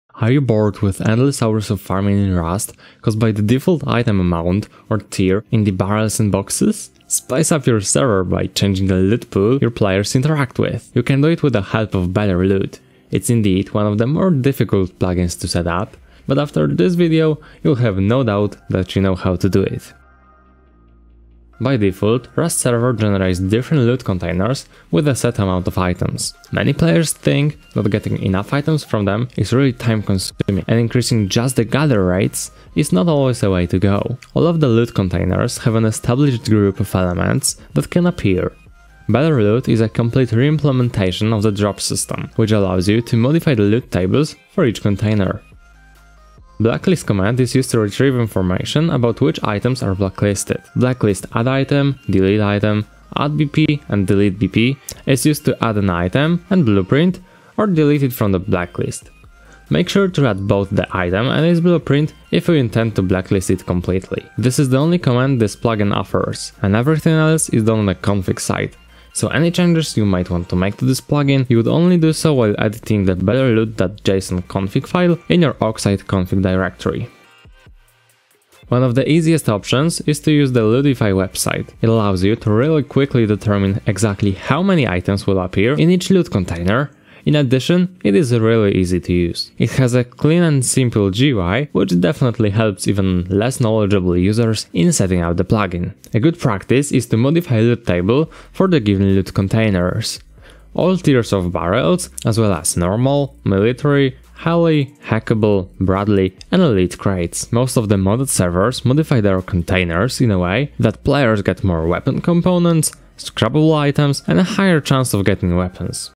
How To Configure Better Loot Oxide Plugin (Rust)
Are you bored with endless hours of farming in Rust caused by the default item amount or tier in the barrels and boxes? Spice up your server by changing the loot pool your players interact with! You can do it with the help of Better Loot! It's indeed one of the more difficult plugins to set up, but after this article you'll have no doubt that you know how to do it.